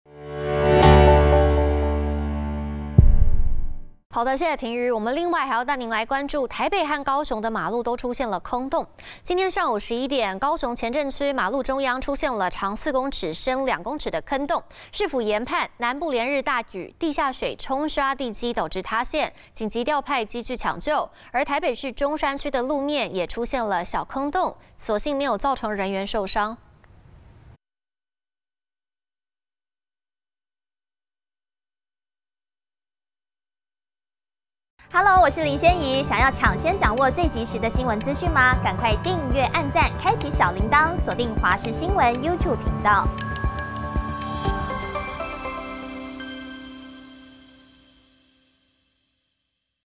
高雄路面驚見天坑！ 封閉車道緊急灌漿搶修｜華視新聞 20230912
地層下陷，意外頻傳！昨(11)日上午11點，高雄前鎮區馬路中央，出現長4公尺、深2公尺的坑洞，汽機車經過全都減速慢行，市府研判南部連日大雨，地下水沖刷地基導致塌陷，緊急調派機具搶救，台北中山區路面，也出現小坑洞，所幸沒有造成人員受傷。